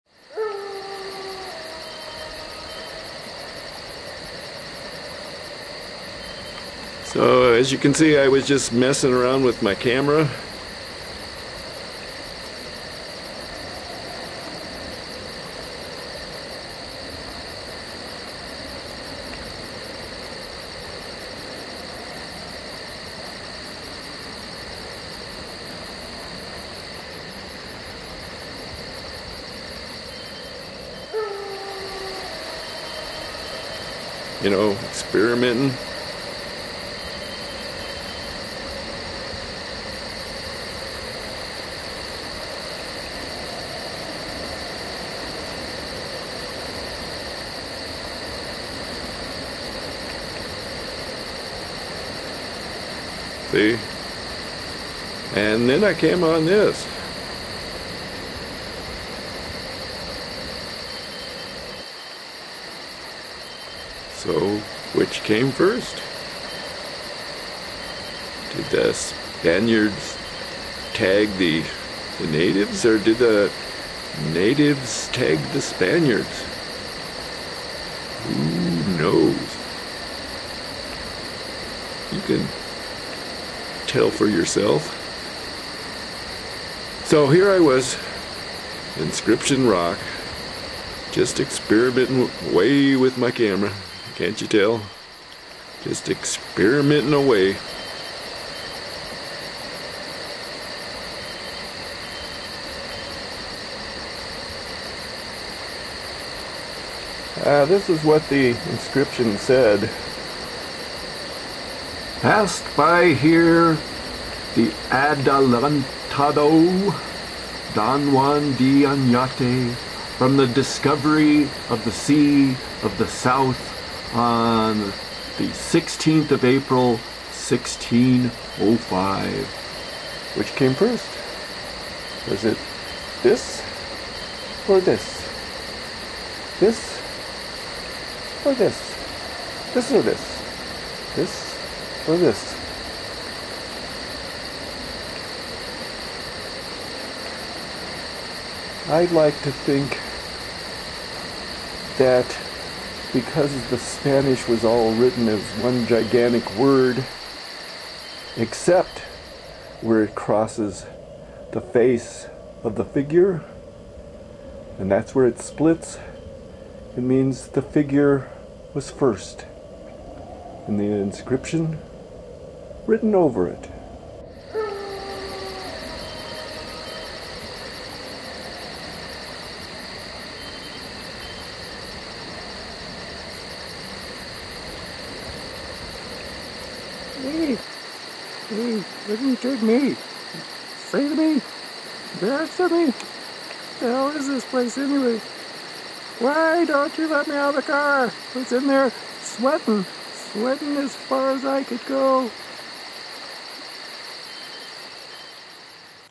El Morro
An experimental movie contemplating the inscriptions. Which came first?